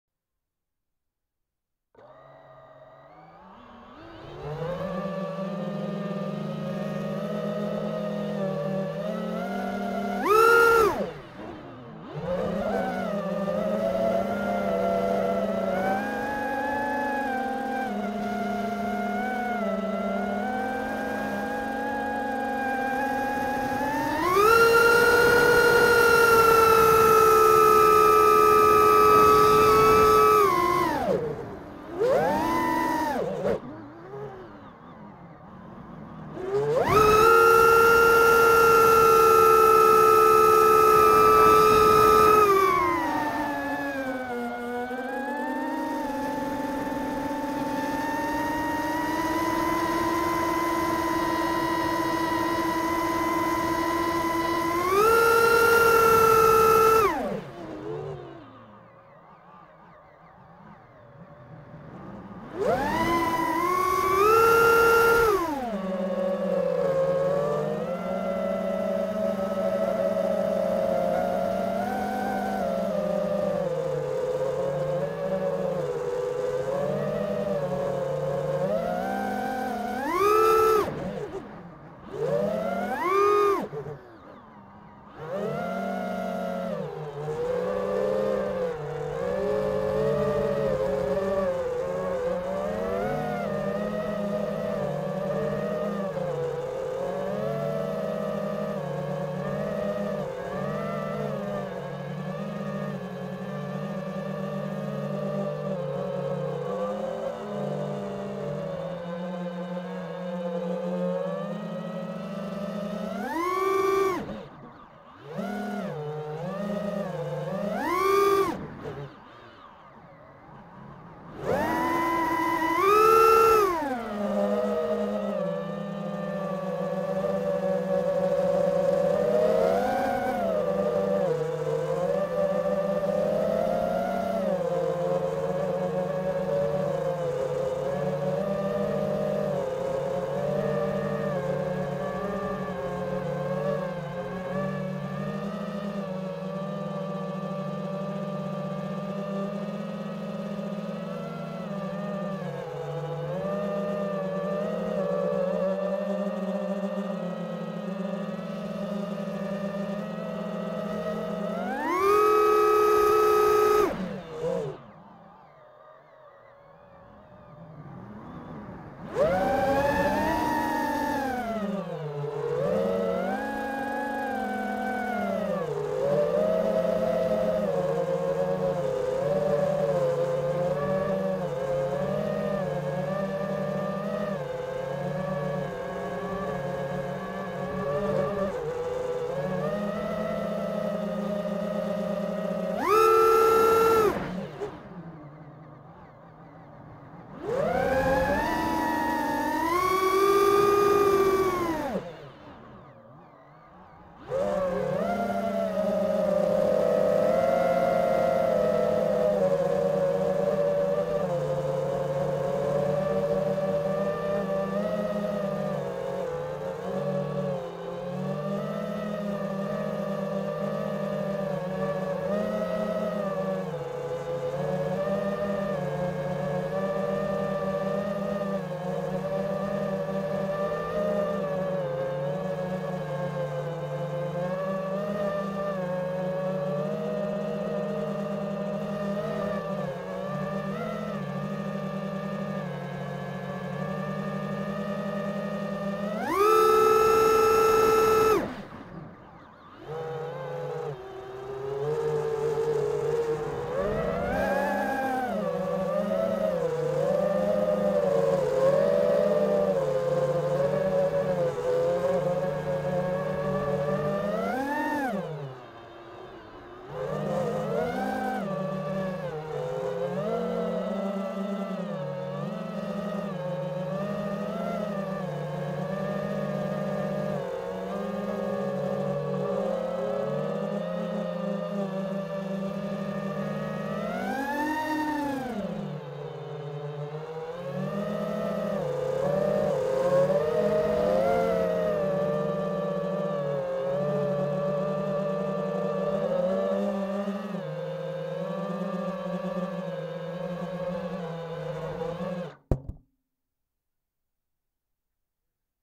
Punishing Tattu Funfly 1550 4S LiPo
I had decided to do a few power loops to evaluate the stamina of the batteries